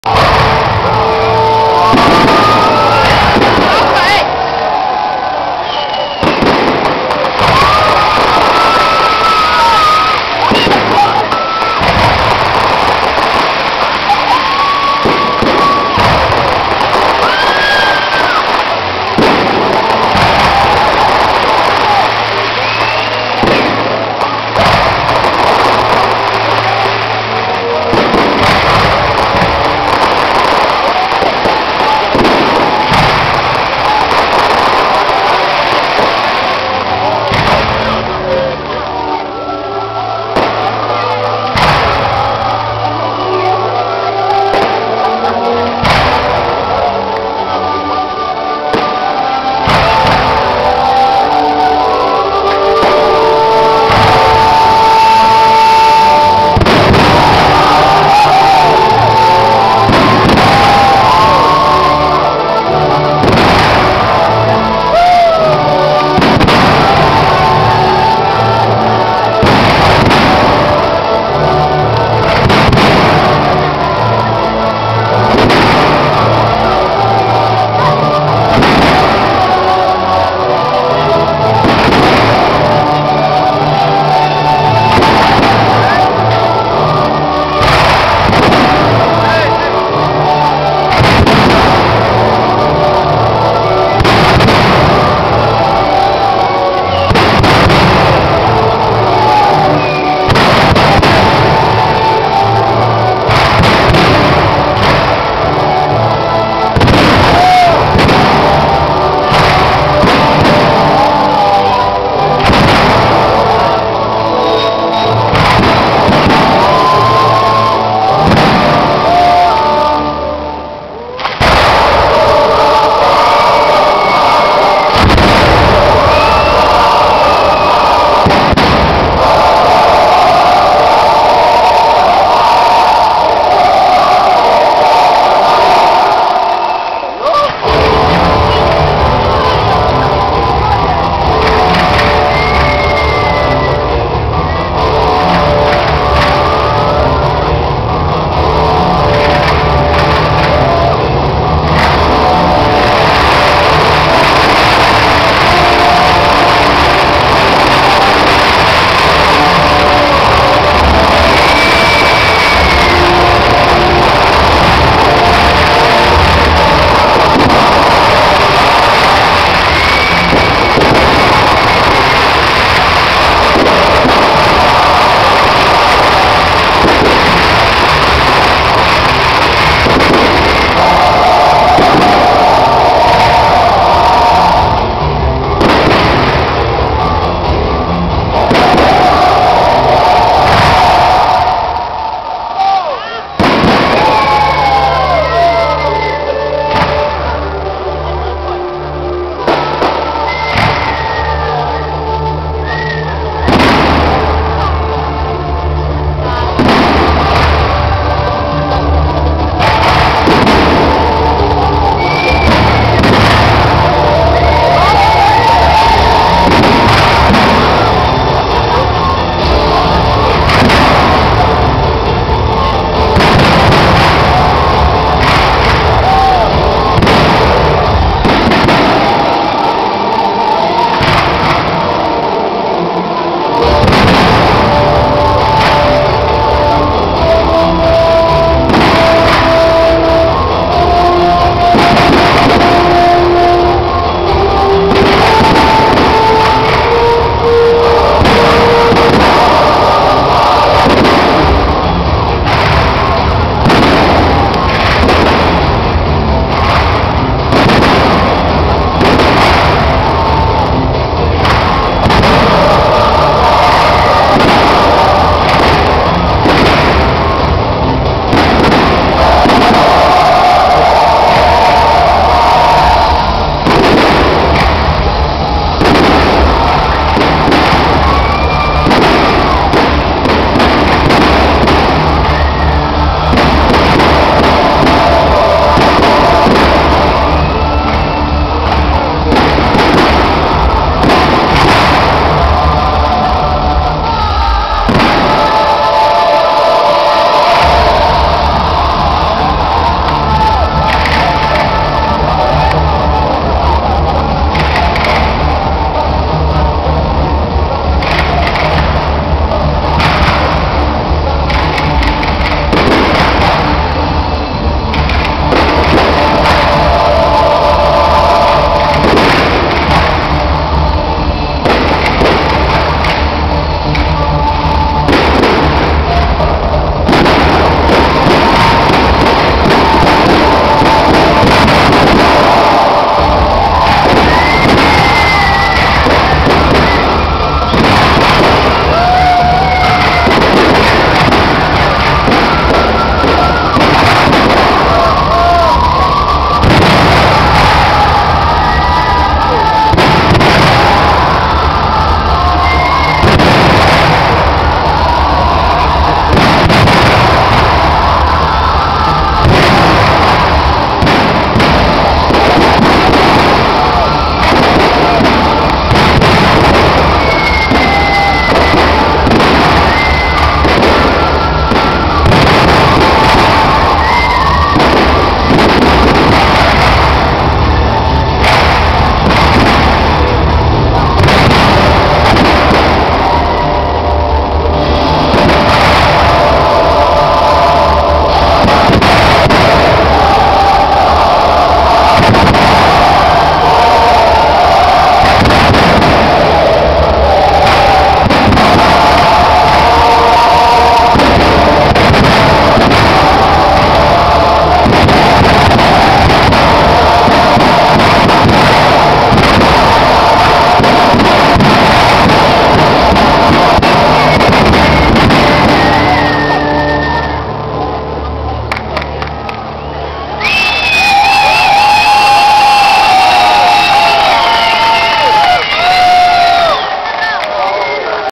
Pokaz sztucznych ogni - Dzień Europy na Bemowie - 9.05.2009
Pokaz sztucznych ogni przygotowany przez firmę Goldregen zakończył koncert z okazji Dnia Europy na Bemowie.